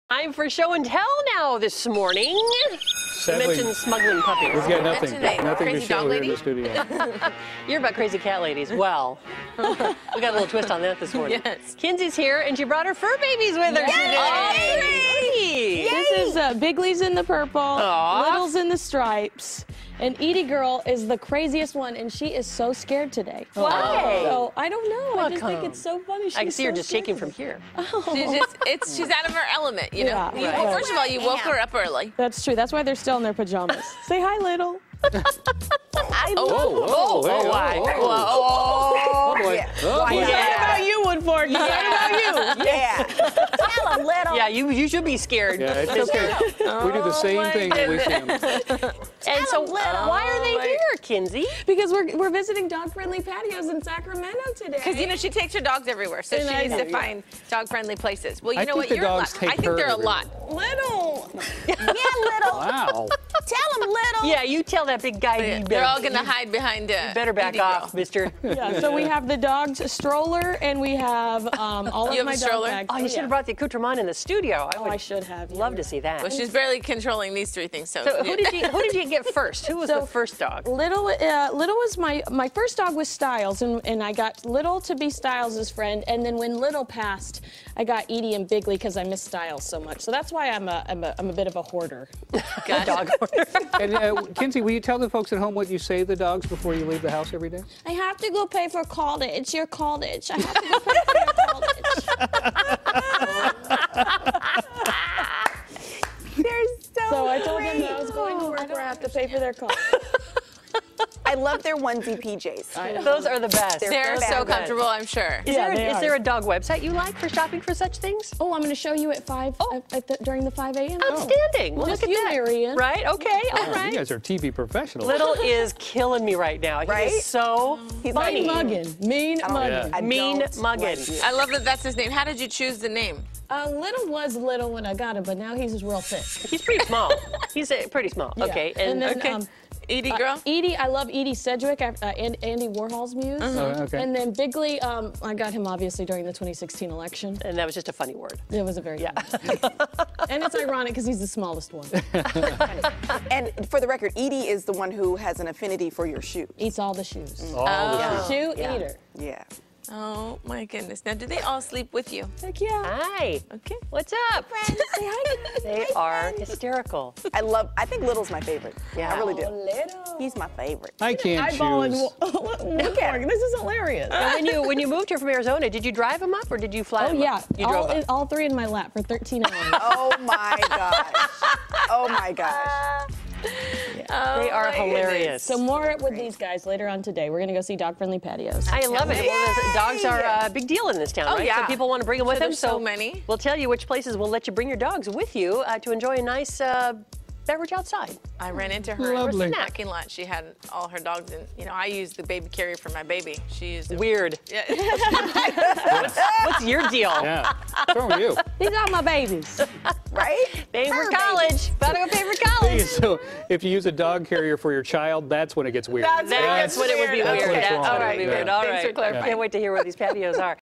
Show & Tell: Kinsey Brings the Dogs
Kinsey brings her three cute dogs into the studio!